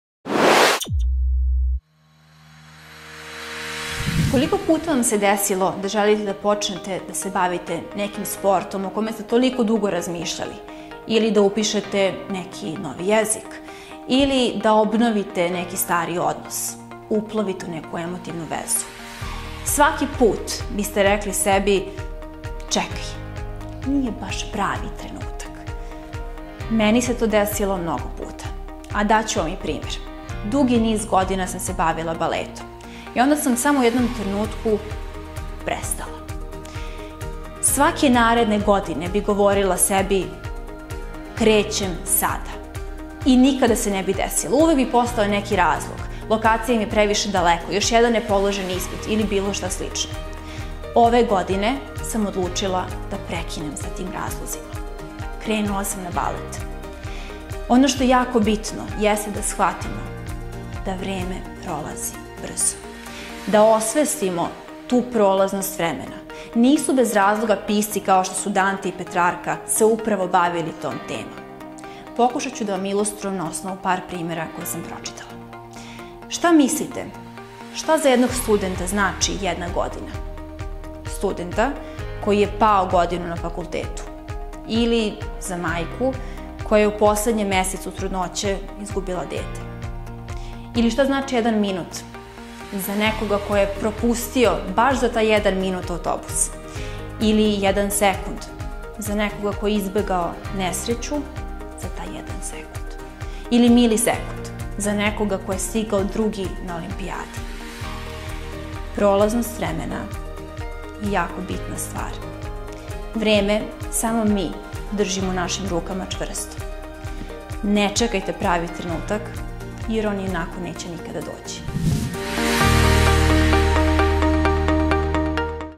Kurs javnog nastupa - Irina Grkinić
Irina Grkinić završila je uvodni kurs javnog nastupa i retorike u Centru za javni govor, na poslednjem času u okviru kursa govorila je na temu "Ne čekajte, ionako nikada neće doći pravi trenutak". Znanja i veštine koje je stekla na kursu primenila je u ovom govoru i poslala snažnu motivacionu poruku.